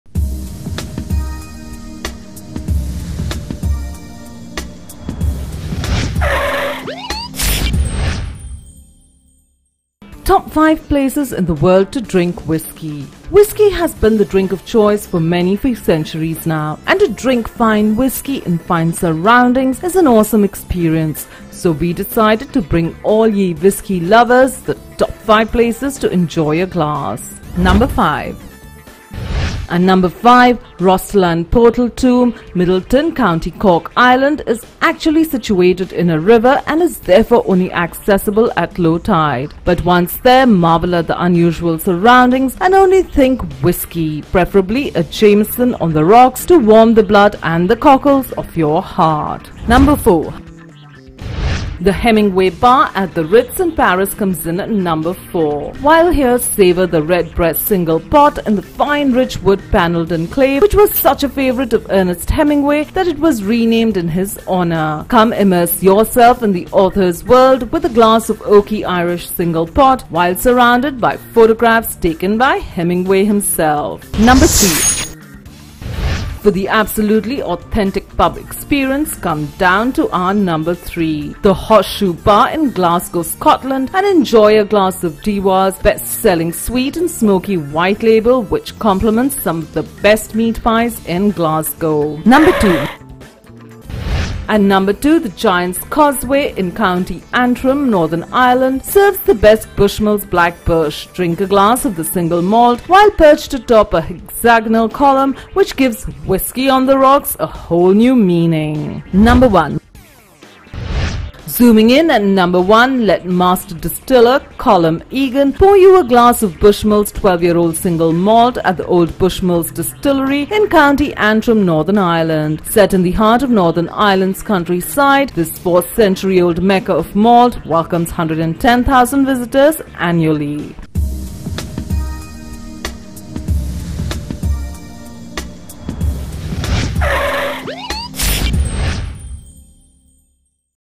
Top 5 Places To Drink Whiskey
Whiskey is a drink which is enjoyed by most of the people around the world. Little do people know where is the best place to get the best whiskey and what is the best place to drink it. Look no further we have all your answers here. For more info watch this video...